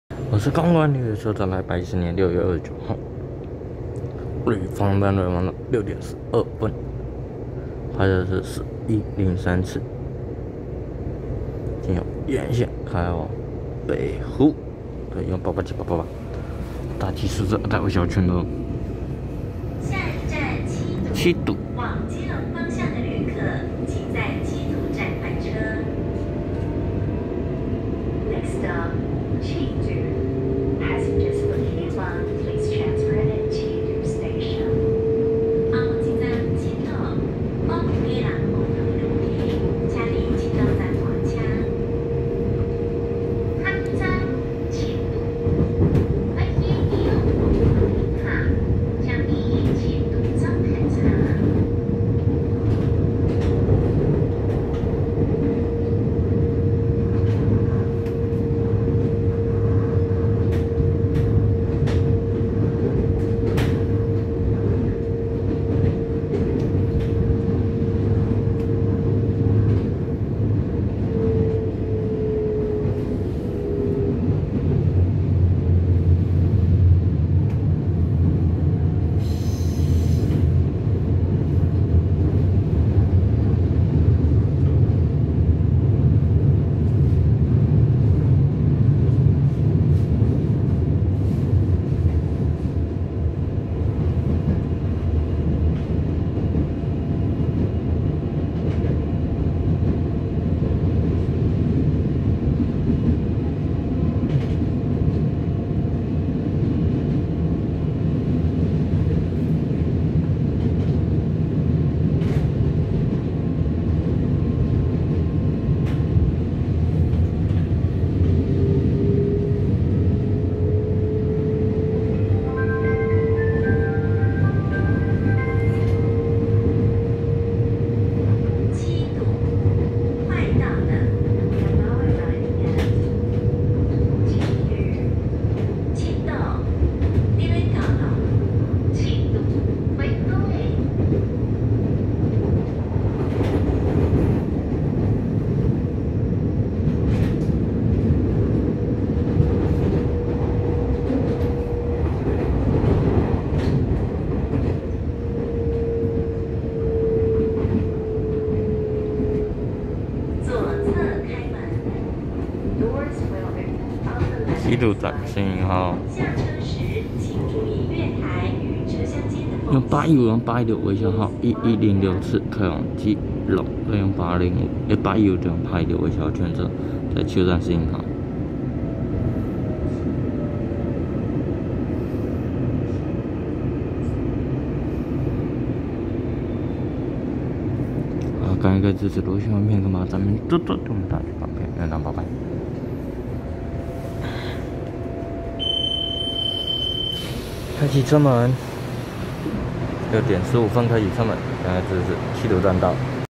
4103次宜蘭發車經由宜蘭線開往北湖的EMU887+EMU888大吉數字二代微笑號區間車，(八堵站→七堵站路程景)，和1106次樹林發車經由西部幹線開往基隆的EMU815+EMU816微笑號區間車
110/6/29八堵站6:12分拍攝4103次宜蘭發車經由宜蘭線開往北湖的EMU887+EMU888大吉數字二代微笑號區間車，(八堵站→七堵站路程景)，和1106次樹林發車經由西部幹線開往基隆的EMU815+EMU816微笑號區間車